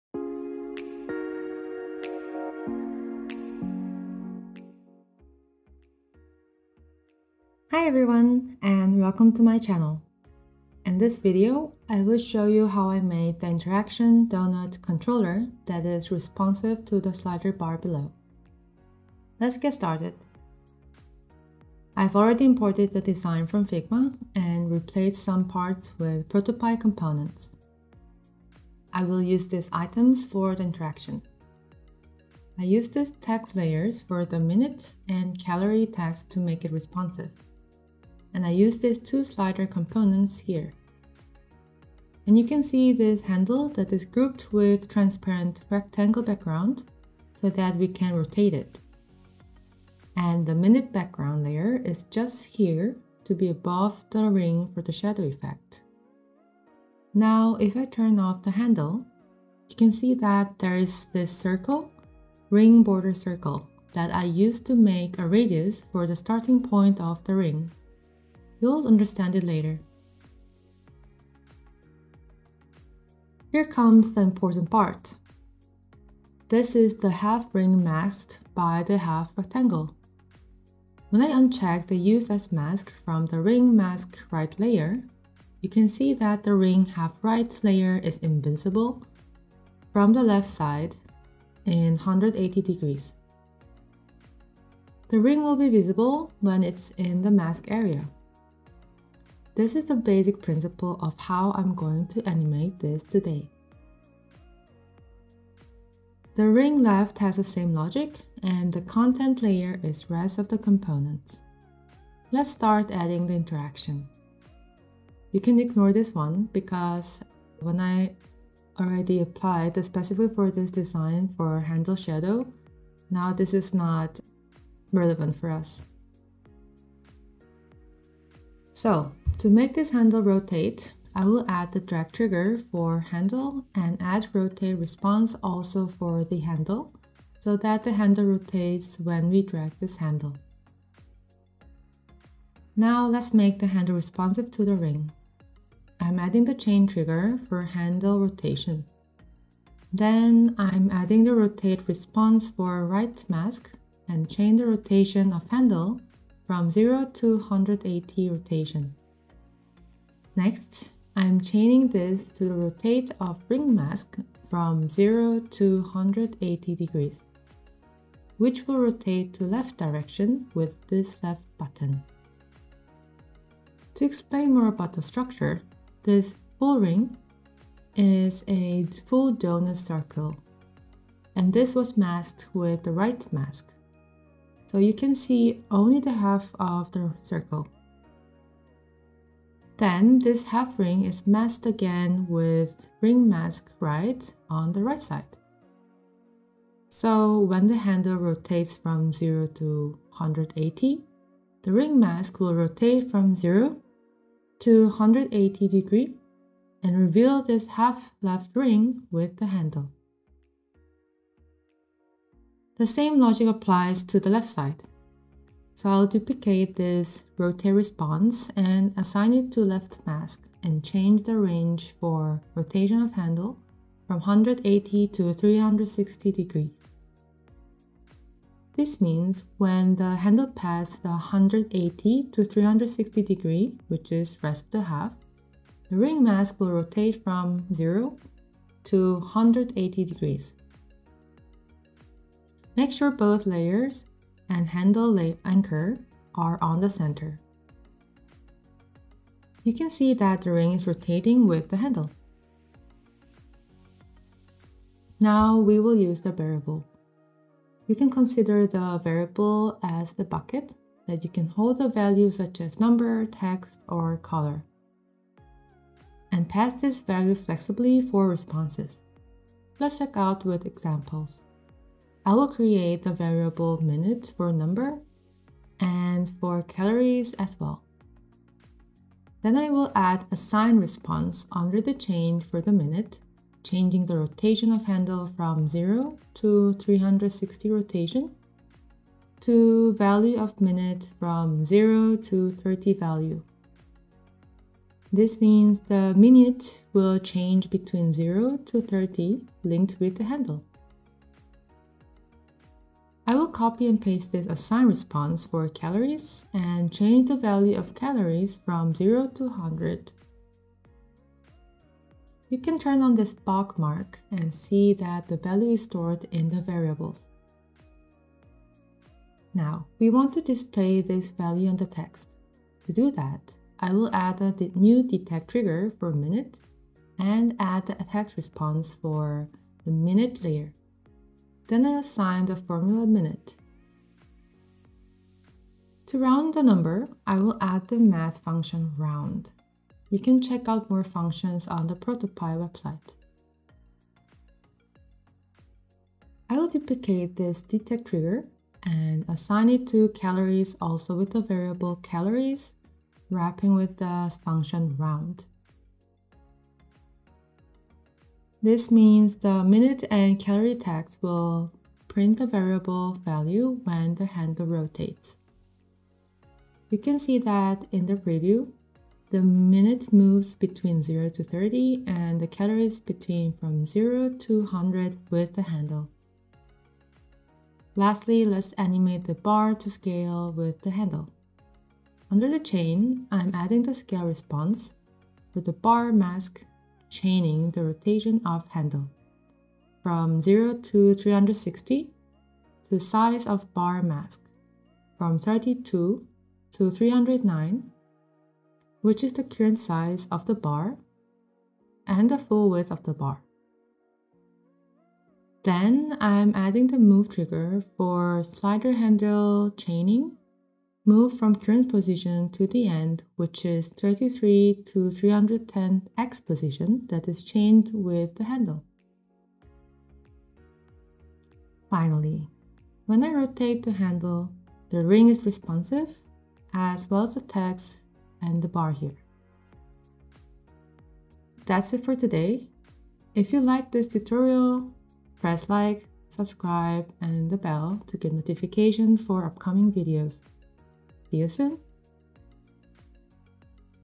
ProtoPie - Rotate Donut Ring Controller (Advance)
In this video,I will show you how I made the interactive donut controller that is responsive to the slider bar below.

🔑 Key feature: Chain, Variable, Detect.